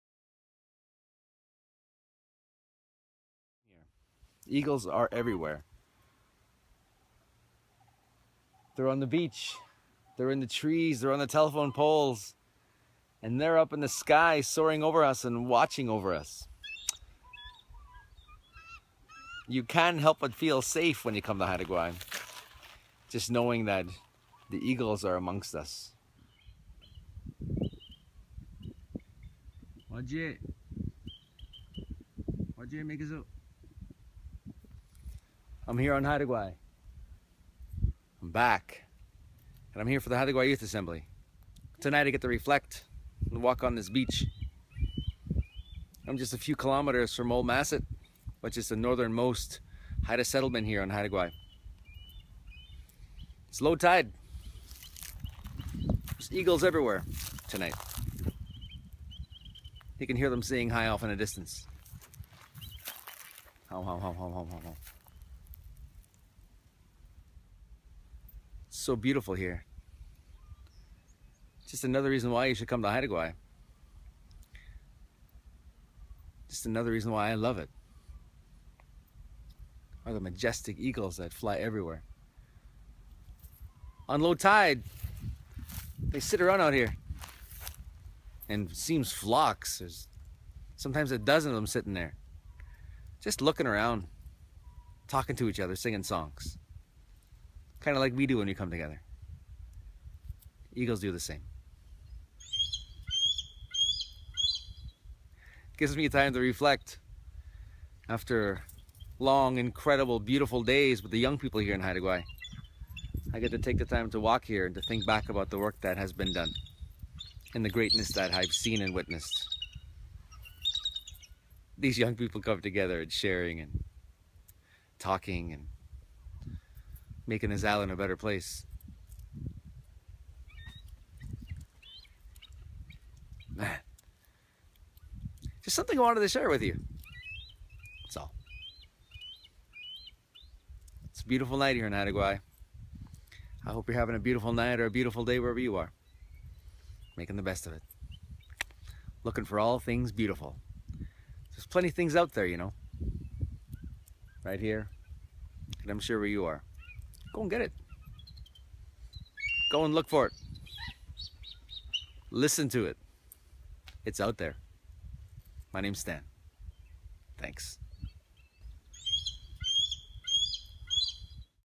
Reflecting Amoungst the Eagles
Taking a moment after day two of the Haida Gwaii Youth Assembly.